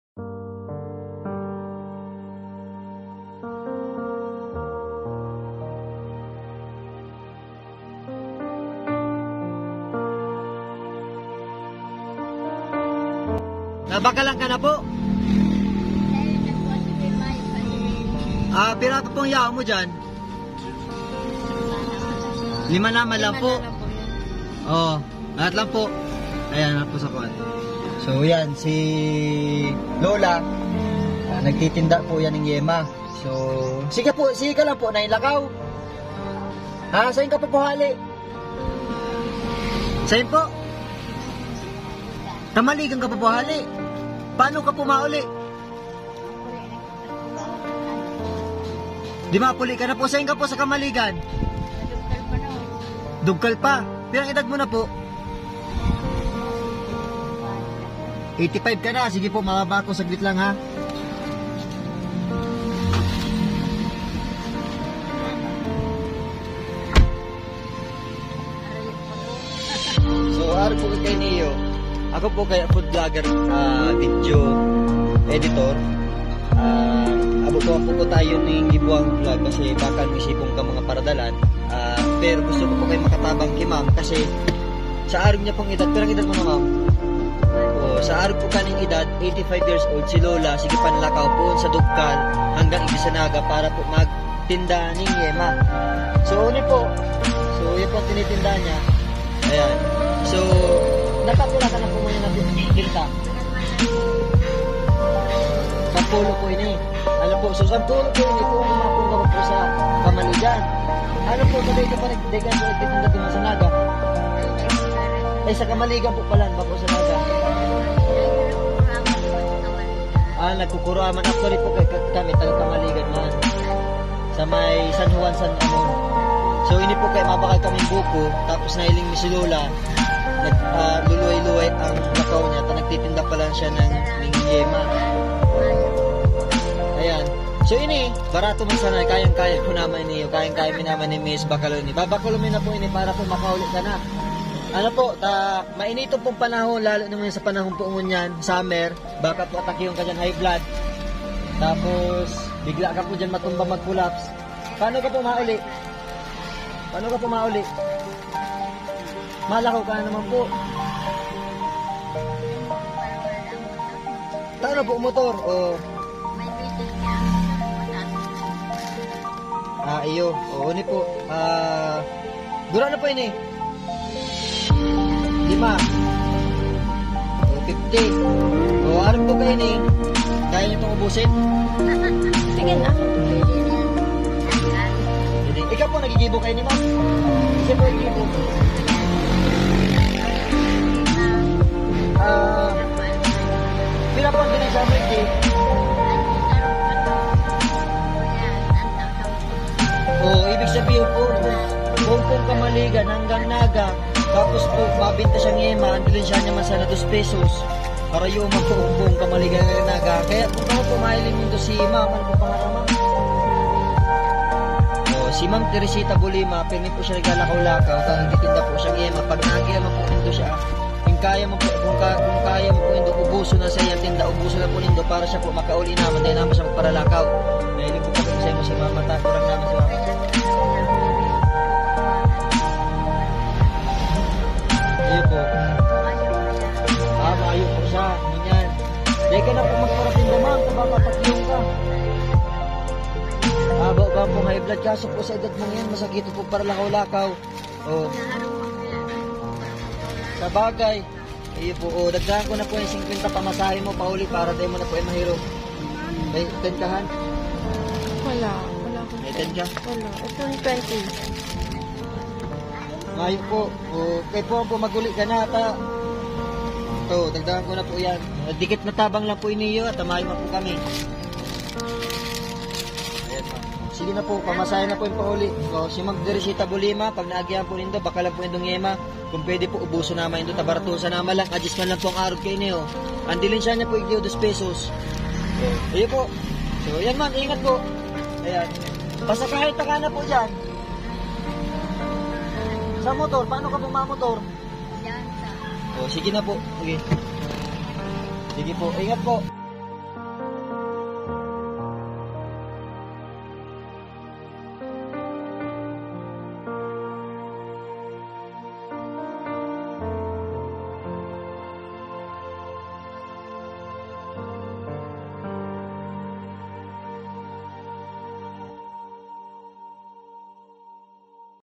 Lola Pabili Yema. 2pesos lang kitaan sa isang pack🙏💯
Lola paratindang "YEMA". 2pesos sana delihinsya sa sarung patos..Pero minalakaw puun Dugcal Camaligan palaog Naga💯🙏 PABAKAL!

Follow and share nalang po ta baka may tumabang man.